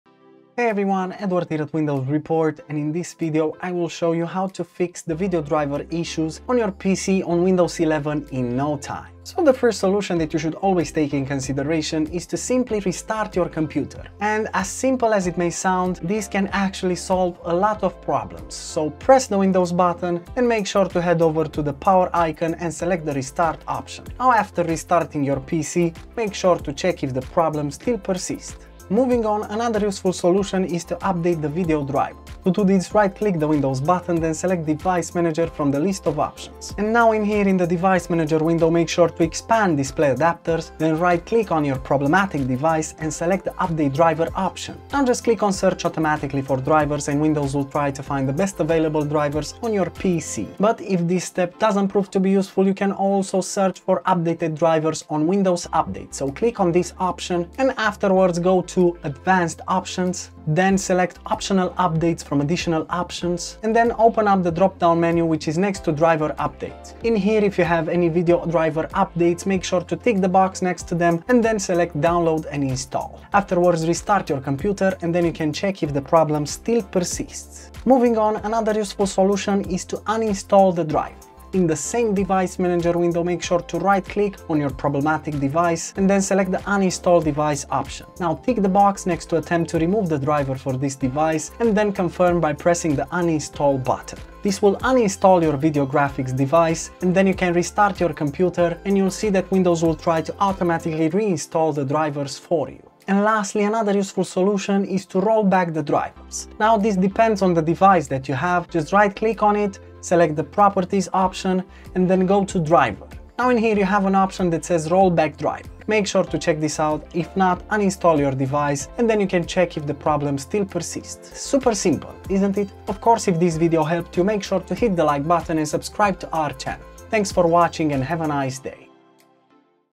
How to Fix the Video Driver Issues in Windows 11 [Easy Guide]
| ⚙️Try Fortect to fix and optimize your PC like a Pro

0:00 - Intro
0:10 - Restart your PC
0:29 - Update your Drivers
1:19 - Uninstall your Drivers